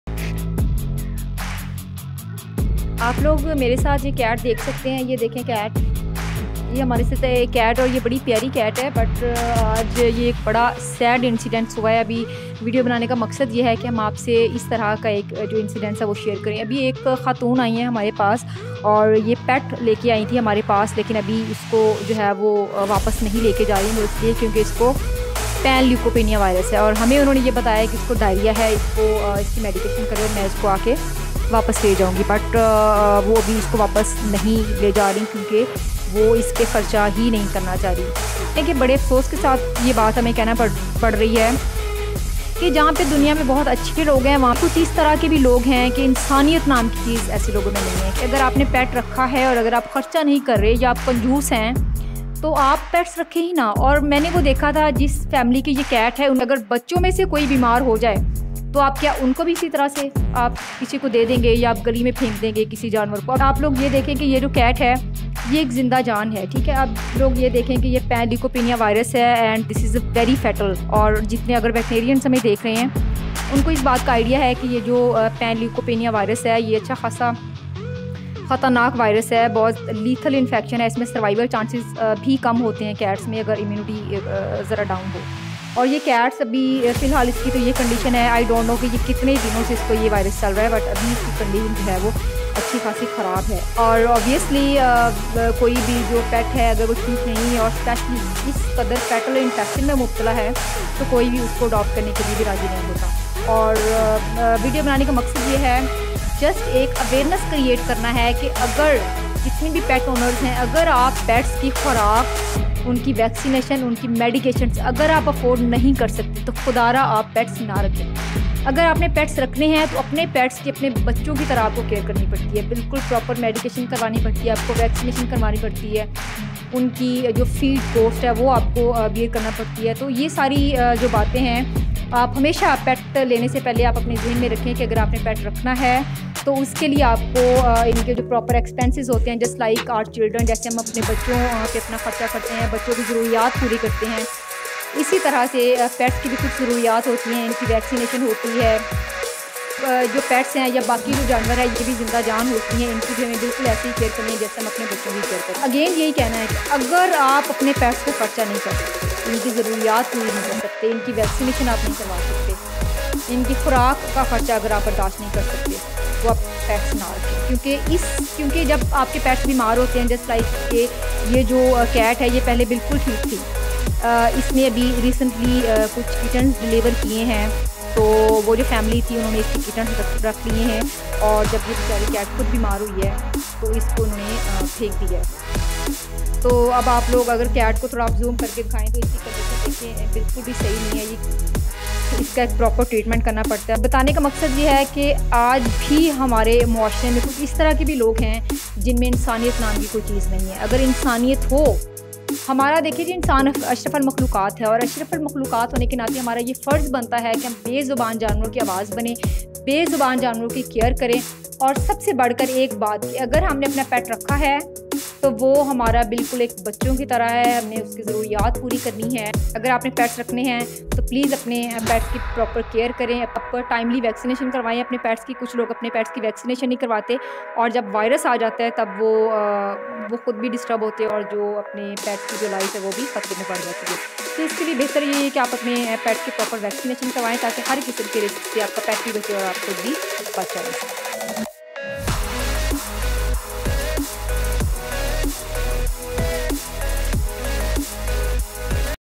Rescue cat|Hearless woman abandoned her cat|owner abandoner her cat|abandoned cat was thrown
heartless woman throw her sick cat at our clinic because she  was sick,
owner abandoned her cat